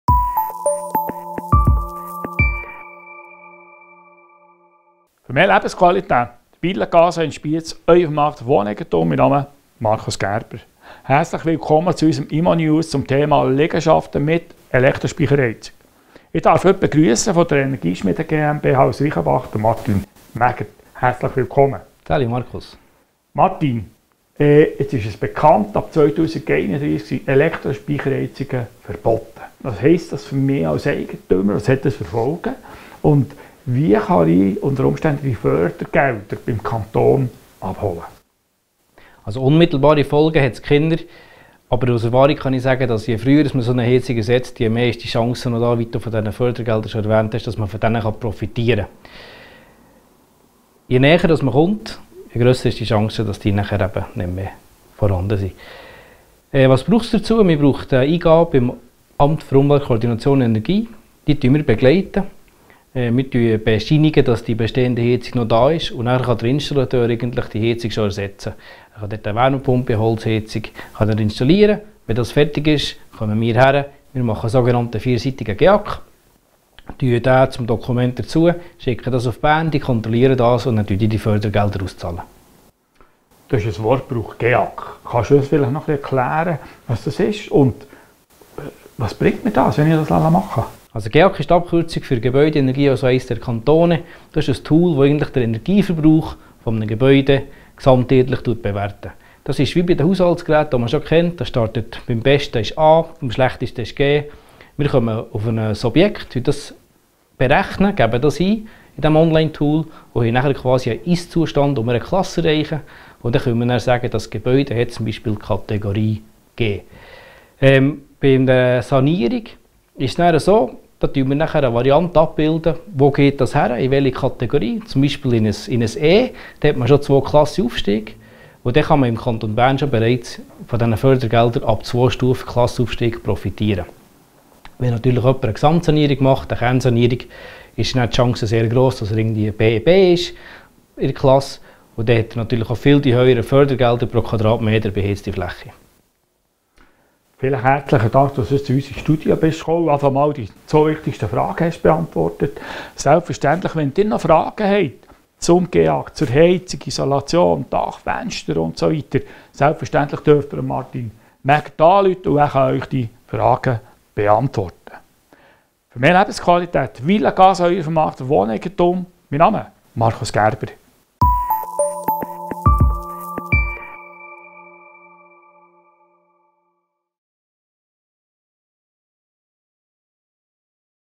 Immonews Elektrospeicherheizung | Informationen Villa Casa AG
Es dreht sich alles um das Thema Elektrospeicherheizung. Das Interview mit Martin Mägert, Inhaber der Energieschmiede GmbH beantwortet die zwei wichtigsten Fragen und steht Ihnen bei weiteren Fragen sehr gerne zur Verfügung.

Energieschmiede GmbH
Martin Mägert
Bälliz 29
3713 Reichenbach

Markus Gerber
Krattigstrasse 2
3700 Spiez